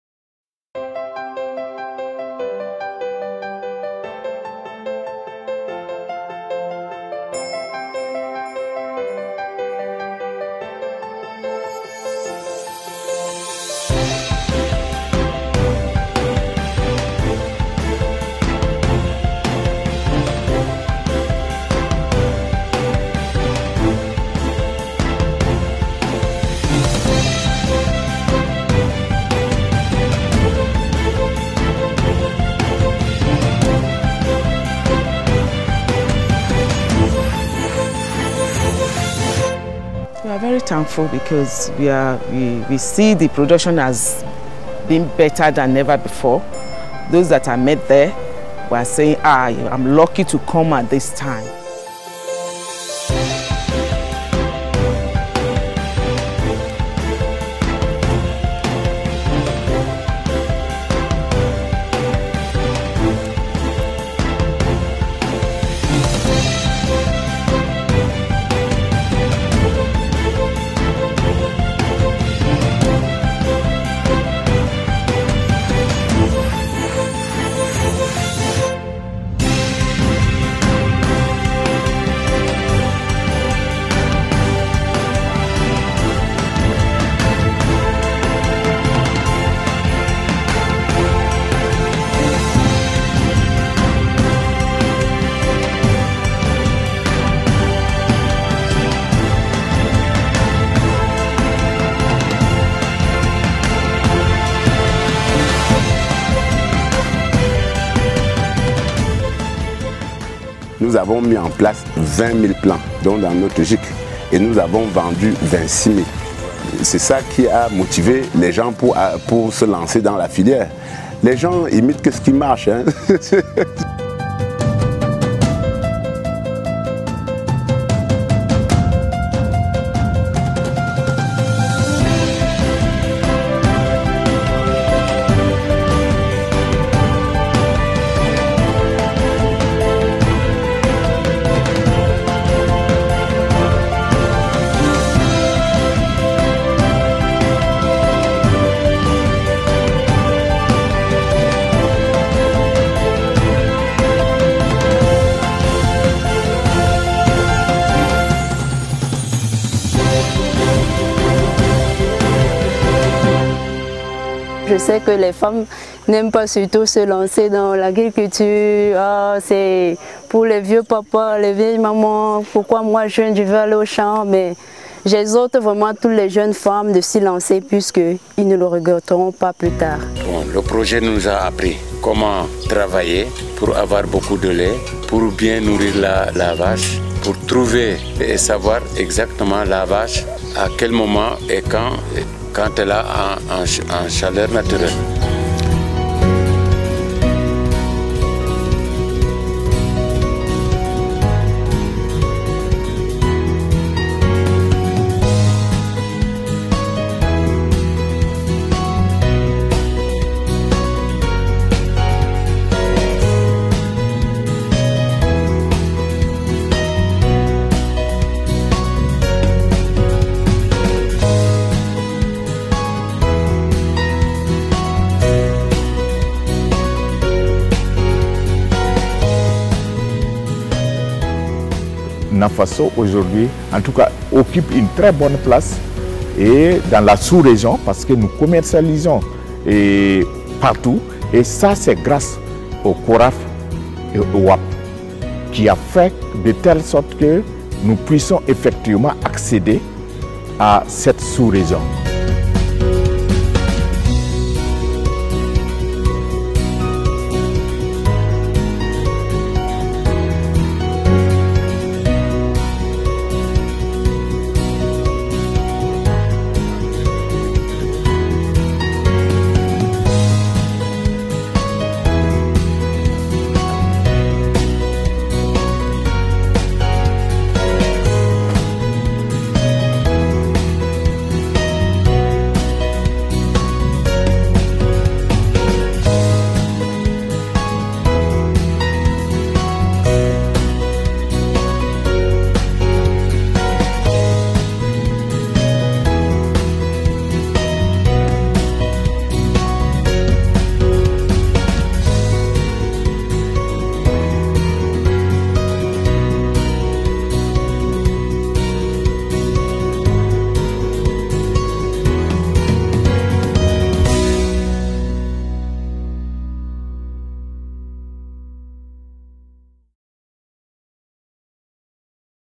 CORAF coordonate agricultural research and development in West and Central Africa
CORAF works for more than 400 million people in 23 countries in West and Central Africa to overcome food and nutrition insecurity through cutting-edge research.